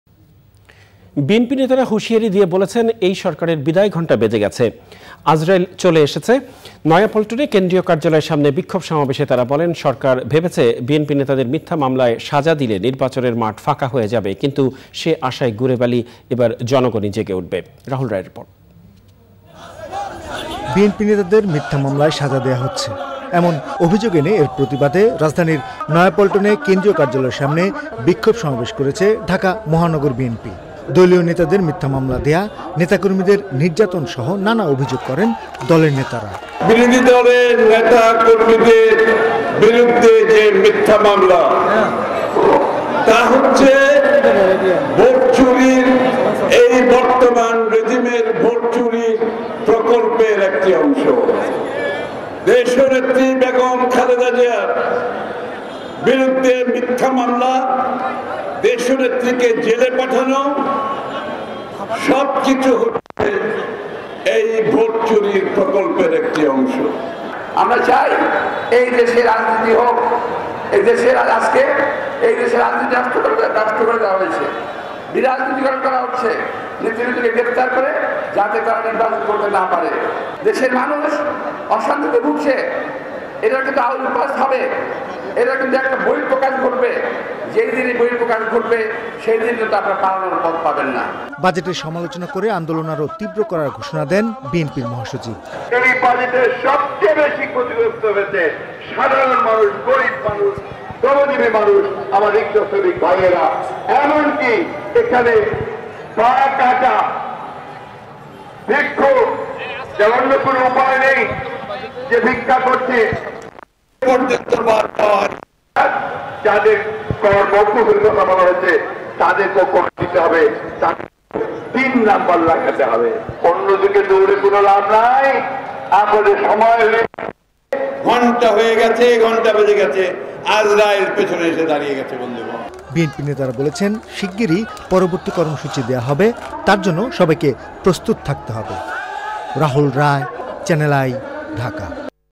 সরকারের বিদায় ঘণ্টা বেজে গেছে, আজরাইল চলে এসেছে
বিএনপি নেতারা হুঁশিয়ারি দিয়ে বলেছেন এই সরকারের বিদায় ঘন্টা বেজে গেছে, আজরাইল চলে এসেছে। নয়া পল্টনে কেন্দ্রীয় কার্যালয়ের সামনে বিক্ষোভ সমাবেশে তারা বলেন সরকার ভেবেছে বিএনপি নেতাদের মিথ্যা মামলায় সাজা দিলে নির্বাচনের মাঠ ফাাঁকা হয়ে যাবে কিন্তু সে আশায় গুড়েবালি এবার জনগণই জেগে উঠবে। রাহুল রায়ের রিপোর্ট।

worldwide.